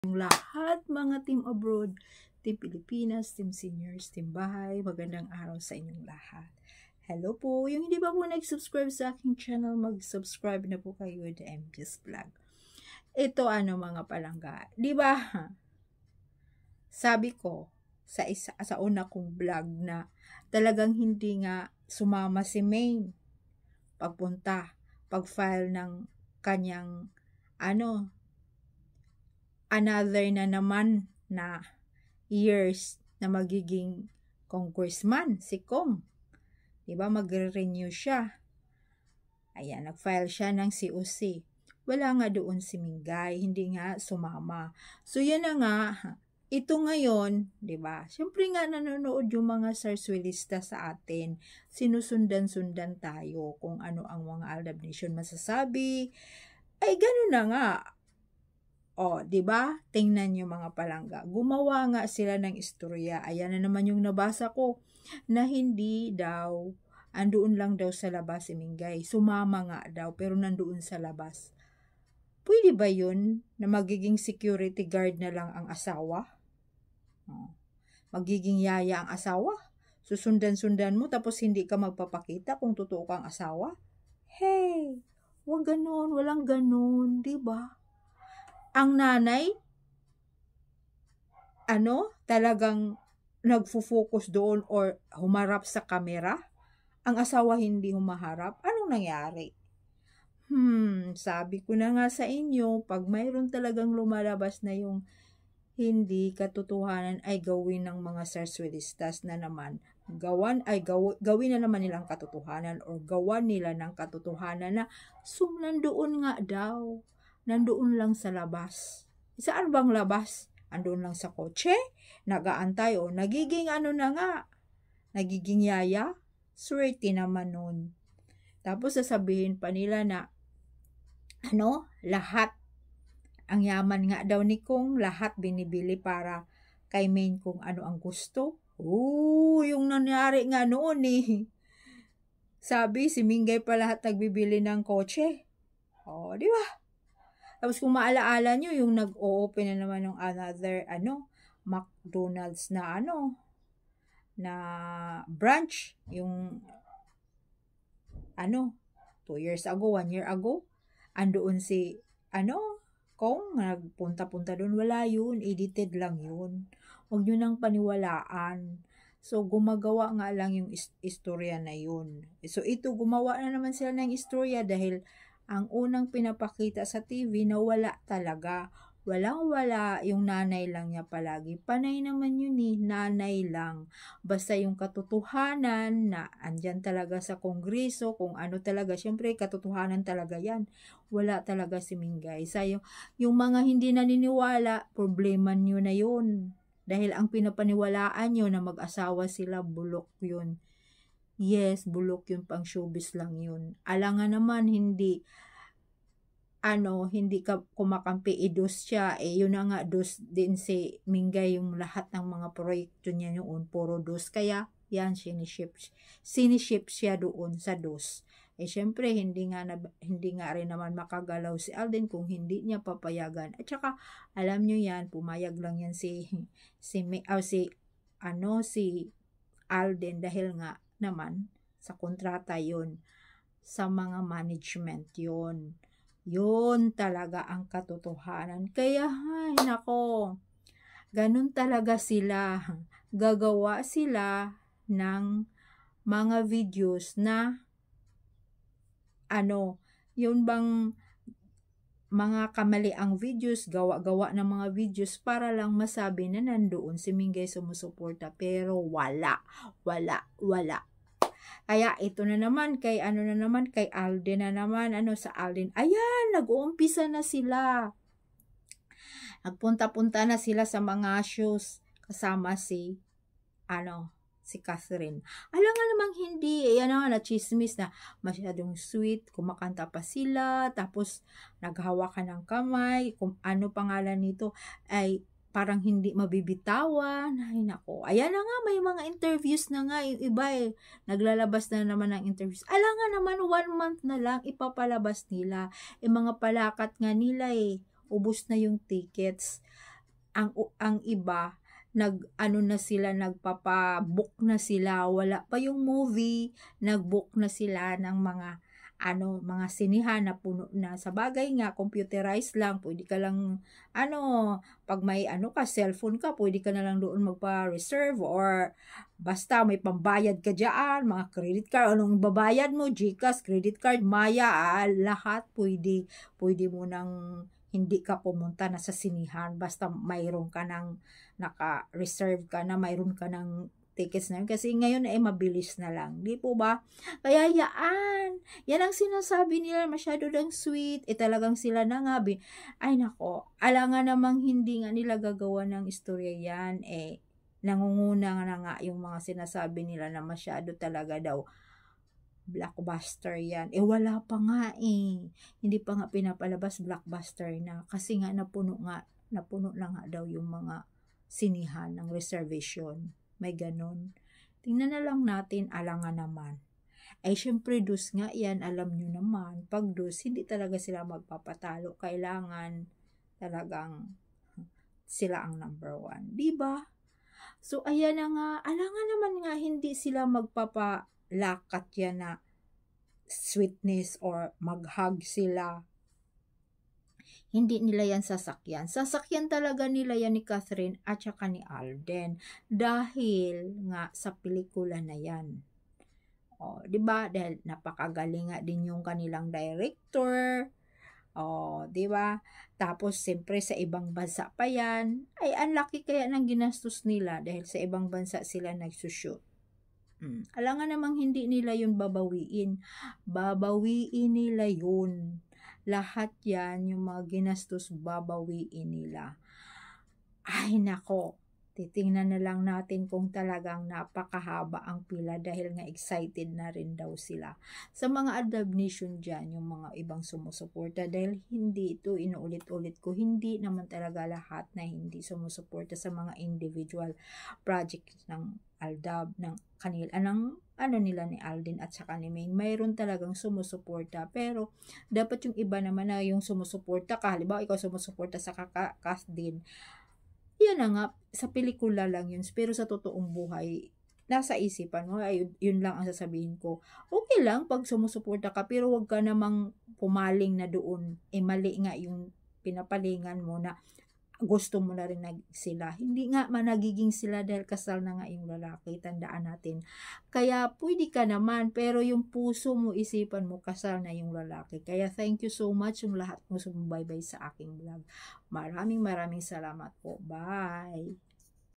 Alden & Maine onscreen sweet bakit my ebidensya na ba my relasyon ang dalawa?alden
Alden & Maine onscreen sweet bakit my ebidensya na ba my relasyon ang dalawa?